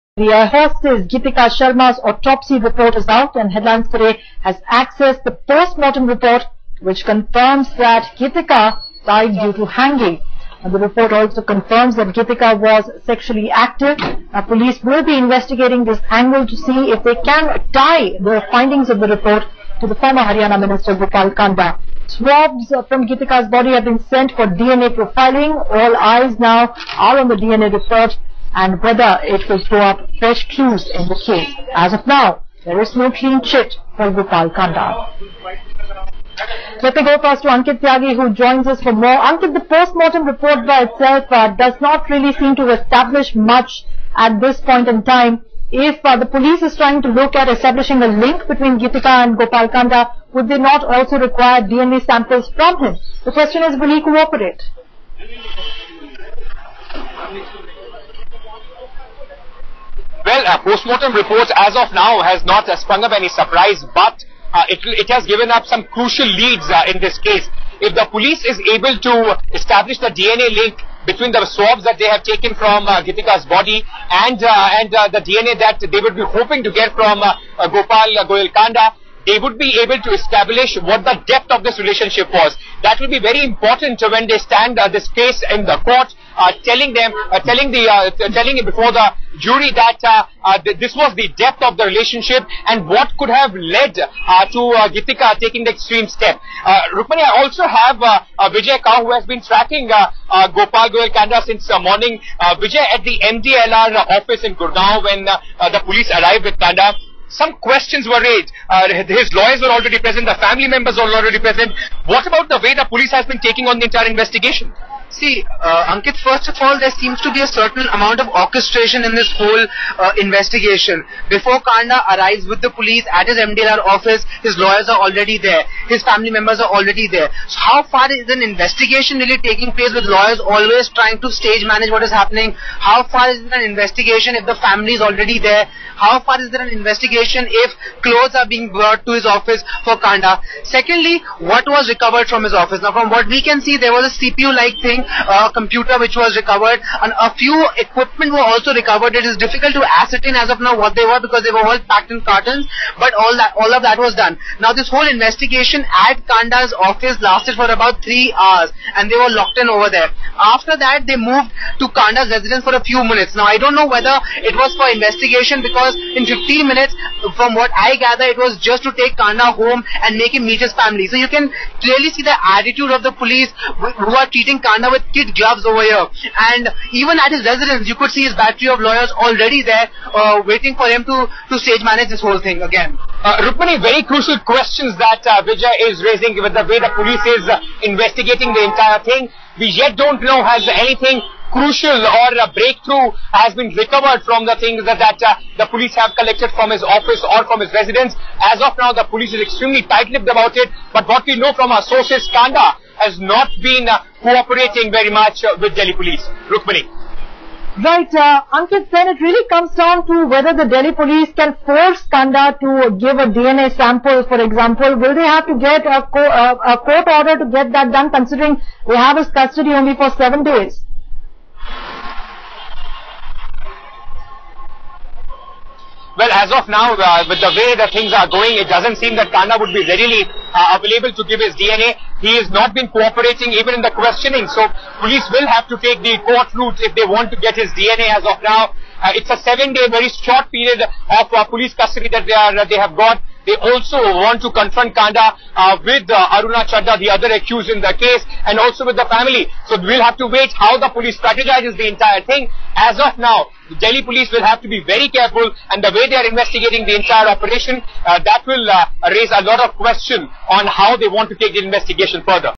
Did Kanda 'sexually assault' Geetika-5
Headlines Today has accessed the post-mortem report of Geetika Sharma.